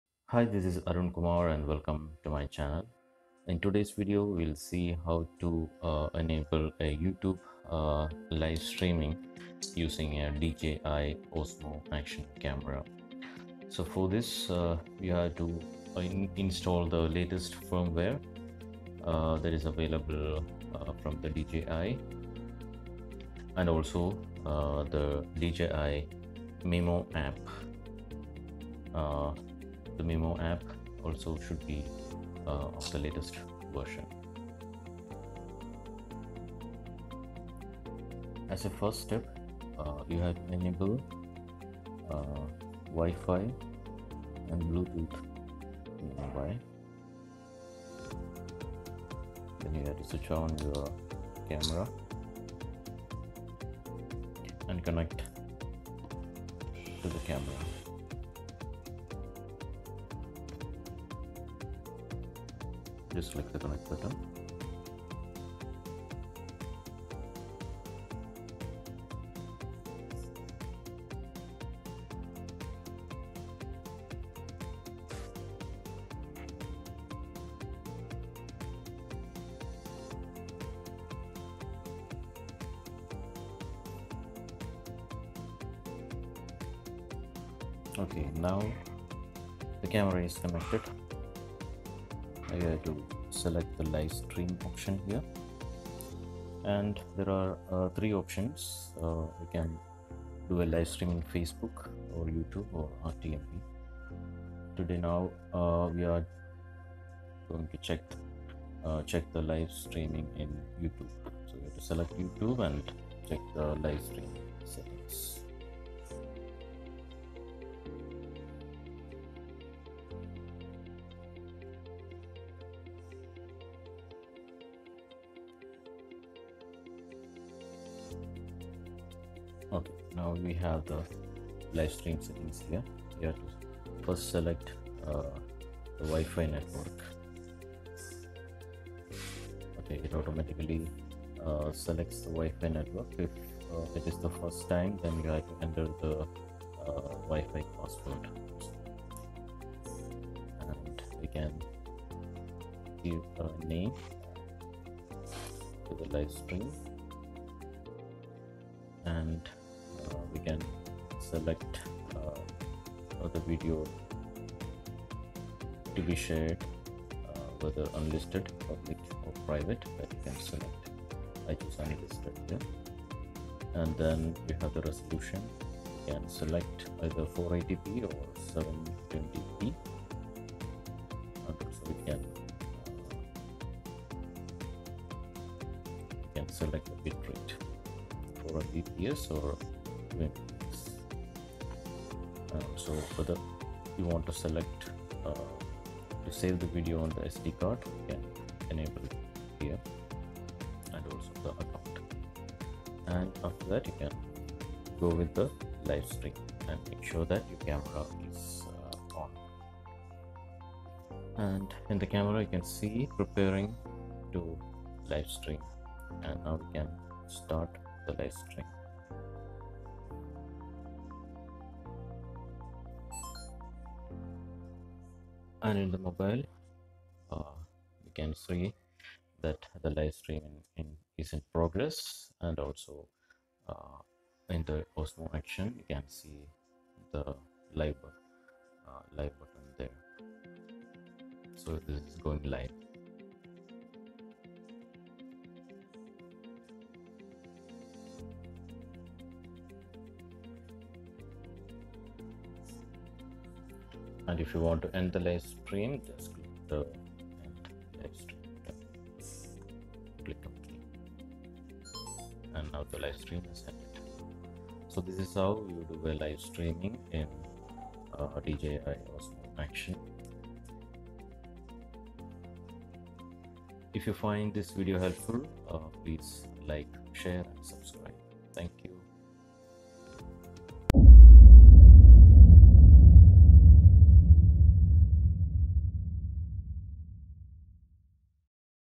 DJI Osmo action | How to live stream to YouTube | New firmware update 2020
How to do a live stream to YouTube

How to download and update Firmware in DJI osmo action using MIMO app

My Mobiles
My Laptops
My Action Cameras
My SLRs

Amazon Prime Paid Membership
Amazon Business
Audible Paid Membership Plan